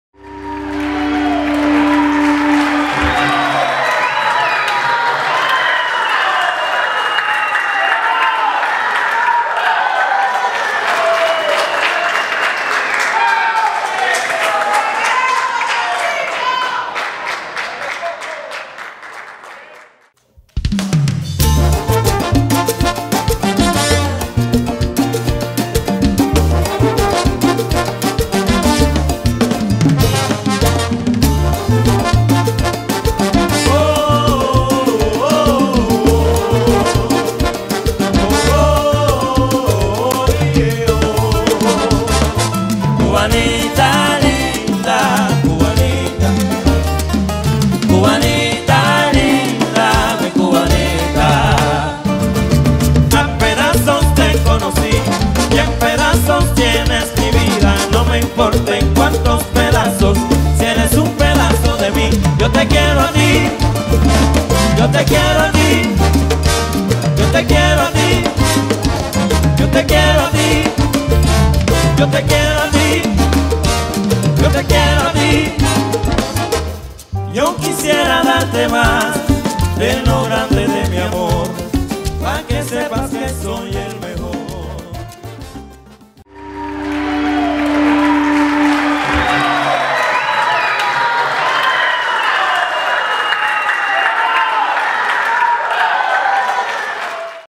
PRESENTACION SUR CARIBE + FRANCISCO PACHECHO Y SU PUEBLO
Presentación de la agrupación cubana Sur Caribe y la agrupación Venezolana Francisco Pacheco y su Pueblo.

Lugar: Ambrosio Oropeza

Barquisimeto-Venezuela